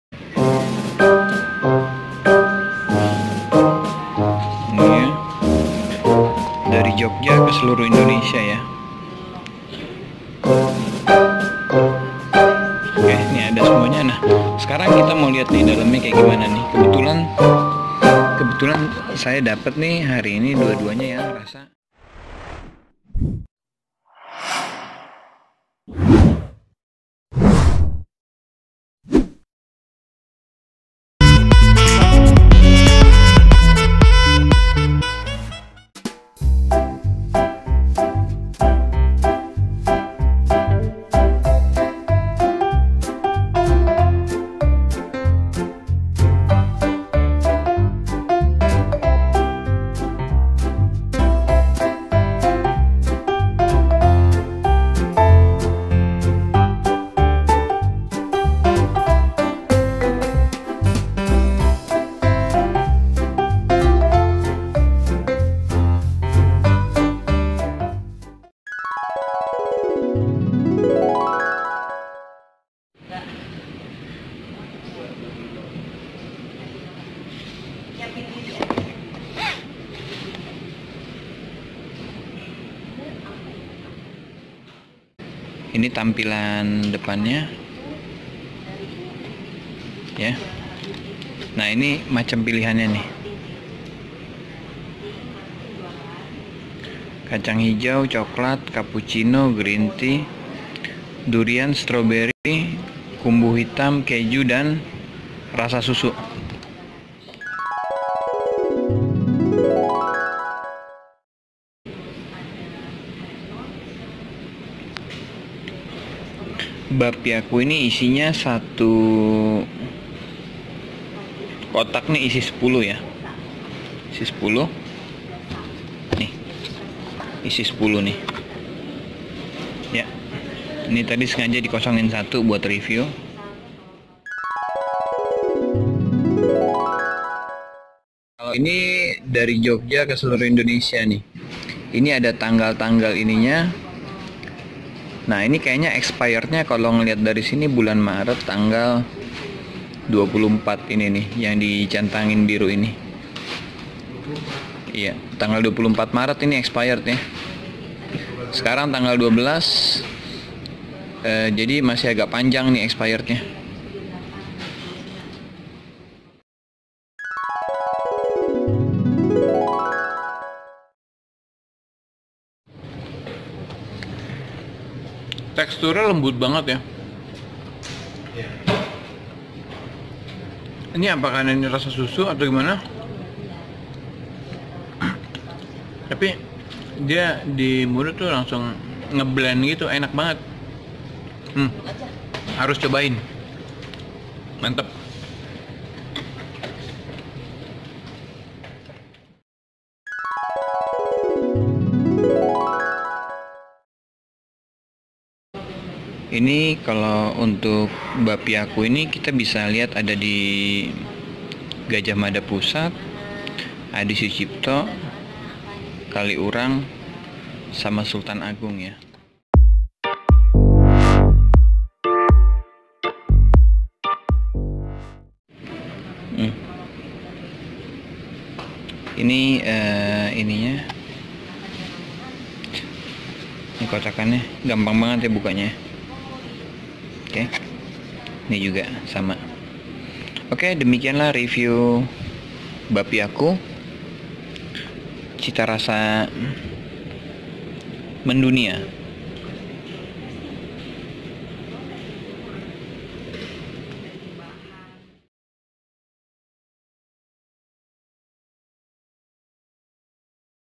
Review BAKPIAKU Special Cita Rasa Mendunia
Review BAKPIAKU Cita Rasa Mendunia

BAKPIAKU cita rasa mendunia ini terbuat dari bahan bahan premium yang di olah secara higienis dan melalui proses pembuatan yang di jaga kualiatasnya.

Seventy Years